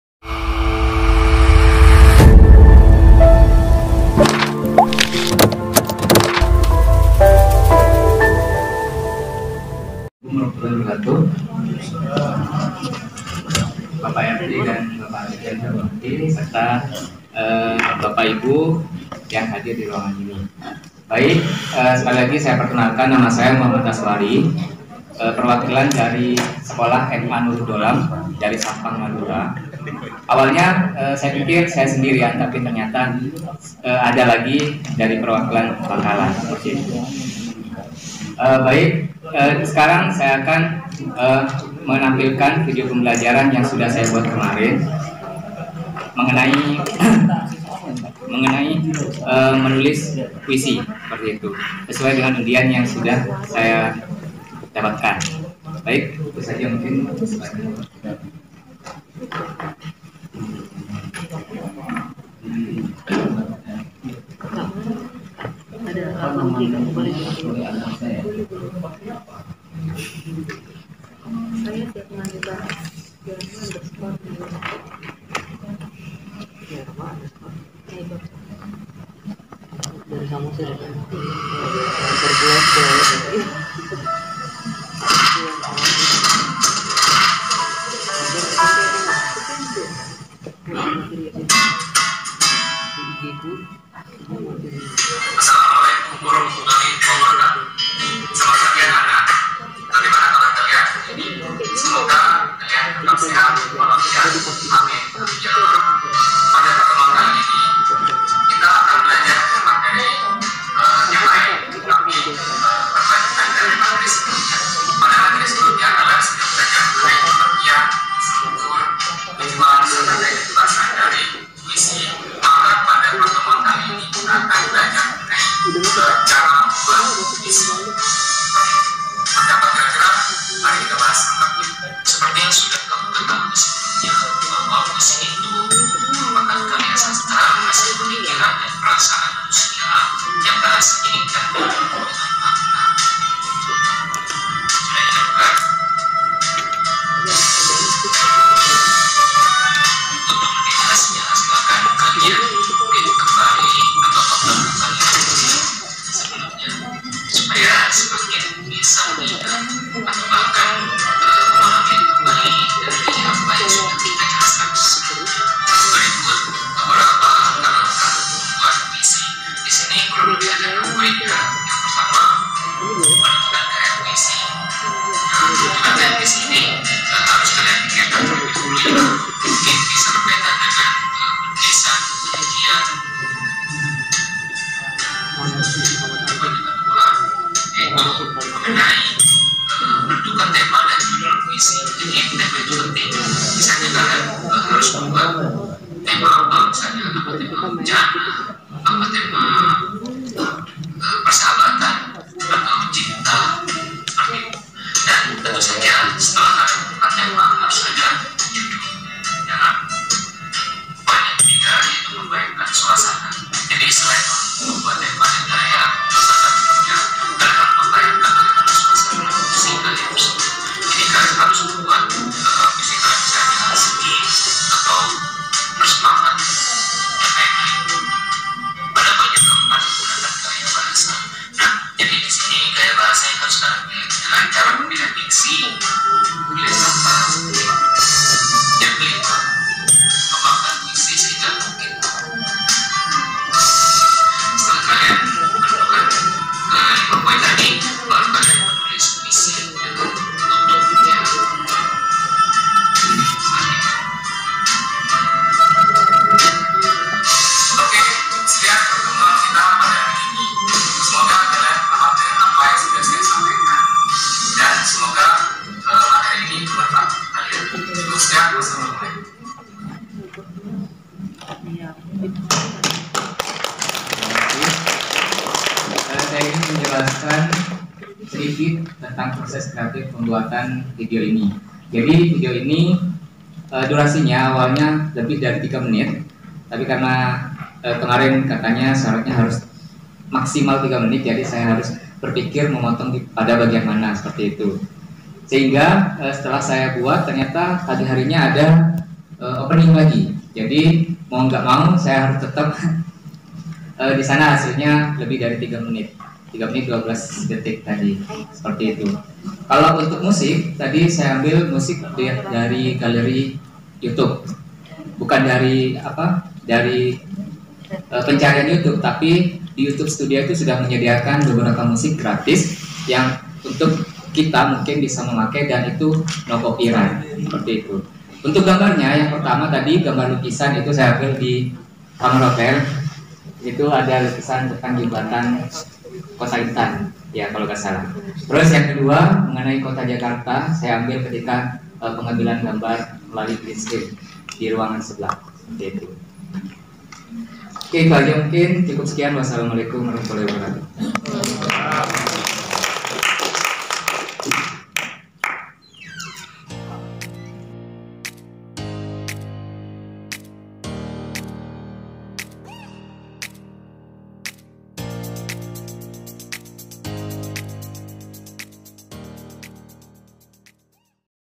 Presentasi Tugas Video Pembelajaran Sastra Berbasis Literasi Digital Tingkat Nasional 2020
Presentasi Tugas Video Pembelajaran Sastra Berbasis Literasi Digital Tingkat Nasional 2020 merupakan video ketika saya melakukan prestasi video pembelajaran sastra yang sudah saya buat dalam kegiatan Bimbingan Teknis Instruktur Pembelajaran Sastra Berbasis Literasi Digital Tingkat Nasional 2020 kemarin di Hotel Novotel Mangga Dua Square Jakarta.

link Video lainnya ketika kegiatan...

Tugas akhir membuat video pembelajaran sastra...